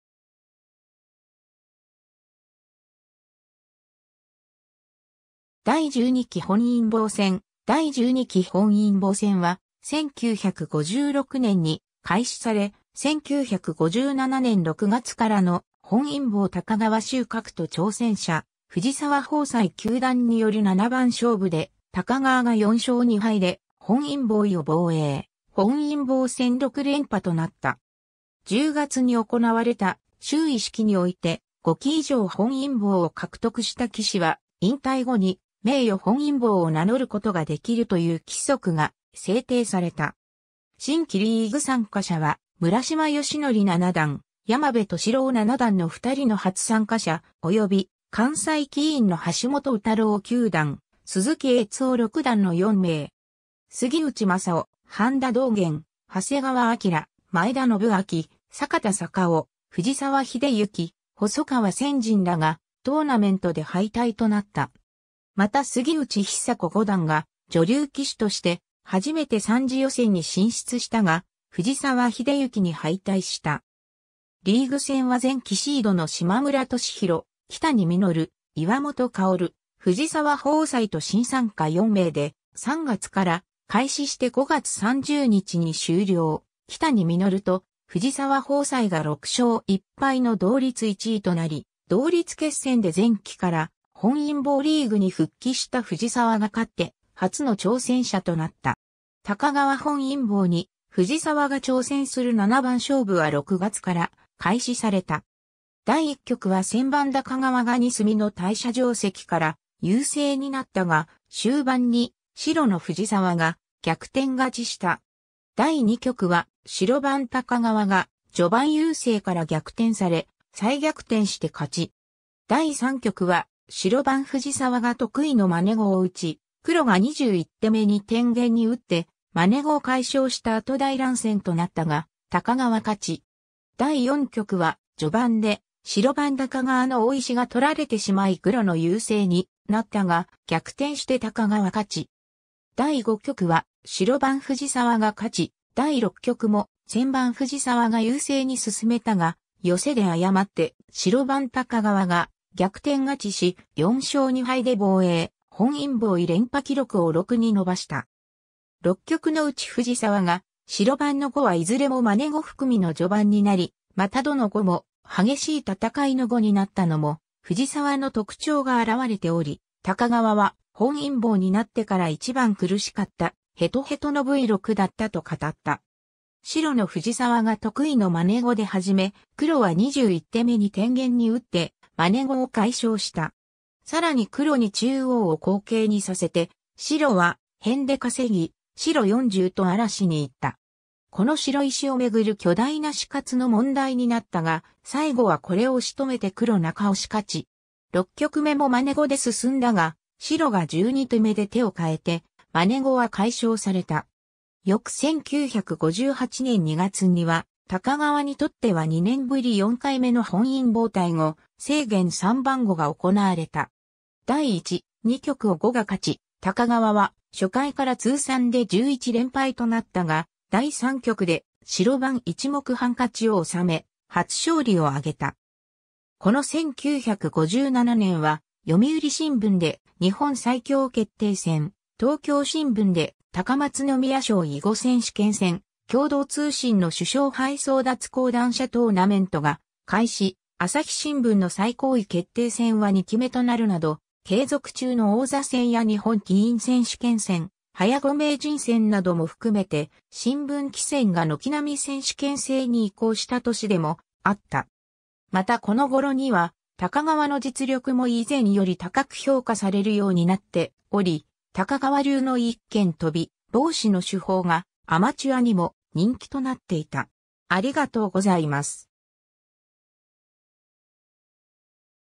第12期本因坊戦
第12期本因坊戦（だい12きほんいんぼうせん） 第12期本因坊戦は、1956年（昭和31年）に開始され、1957年6月からの本因坊高川秀格と挑戦者藤沢朋斎九段による七番勝負で、高川が4勝2敗で本因坊位を防衛、本因坊戦6連覇となった。
10月に行われた就位式において、5期以上本因坊を獲得した棋士は引退後に名誉本因坊を名乗ることができるという規則が制定された。
 新規リーグ参加者は、村島誼紀七段、山部俊郎七段の二人の初参加者、及び関西棋院の橋本宇太郎九段、鈴木越雄六段の4名。
杉内雅男、半田道玄、長谷川章、前田陳爾、坂田栄男、藤沢秀行、細川千仭らがトーナメントで敗退となった。
また杉内寿子五段が女流棋士として初めて3次予選に進出したが、藤沢秀行に敗退した。
 リーグ戦は前期シードの島村利博、木谷實、岩本薫、藤沢朋斎と新参加4名で、3月から開始して5月30日に終了、木谷實と藤沢朋斎が6勝1敗の同率1位となり、同率決戦で前期から本因坊リーグに復帰した藤沢が勝って、初の挑戦者となった。
 高川本因坊に藤沢が挑戦する七番勝負は6月から開始された。
第1局は先番高川が2隅の大斜定石から優勢になったが、終盤に白の藤沢が逆転勝ちした。
第2局は白番高川が、序盤優勢から逆転され、再逆転して勝ち。
第3局は白番藤沢が得意のマネ碁を打ち、黒が21手目に天元に打ってマネ碁を解消した後大乱戦となったが高川勝ち。
第4局は序盤で白番高川の大石が取られてしまい黒の優勢になったが、逆転して高川勝ち。
第5局は白番藤沢が勝ち、第6局も先番藤沢が優勢に進めたが、ヨセで誤って白番高川が逆転勝ちし、4勝2敗で防衛、本因坊位連覇記録を6に伸ばした。
 6局のうち藤沢が白番の碁はいずれもマネ碁含みの序盤になり、またどの碁も激しい戦いの碁になったのも藤沢の特徴が現れており、高川は「本因坊になってから一番苦しかった」「ヘトヘトのV6だった」と語った。
 白の藤沢が得意のマネ碁で始め、黒は21手目に天元に打ってマネ碁を解消した。
さらに黒に中央を好形にさせて白は辺で稼ぎ、白40と荒らしに行った。
この白石をめぐる巨大な死活の問題になったが、最後はこれを仕留めて黒中押し勝ち。
6局目もマネ碁で進んだが、白が12手目で手を変えてマネ碁は解消された。
 翌1958年2月には、高川にとっては2年ぶり4回目の本因坊対呉清源三番碁が行われた。
第1、2局を呉が勝ち、高川は初回から通算で11連敗となったが、第3局で白番1目半勝ちを収め、初勝利を挙げた。
 この1957年は、読売新聞で日本最強決定戦、東京新聞で高松宮賞囲碁選手権戦、共同通信の首相杯争奪高段者トーナメントが開始、朝日新聞の最高位決定戦は2期目となるなど、継続中の王座戦や日本棋院選手権戦、早碁名人戦なども含めて新聞棋戦が軒並み選手権制に移行した年でもあった。
またこの頃には高川の実力も以前より高く評価されるようになっており、高川流の一間飛び、ボウシの手法がアマチュアにも人気となっていた。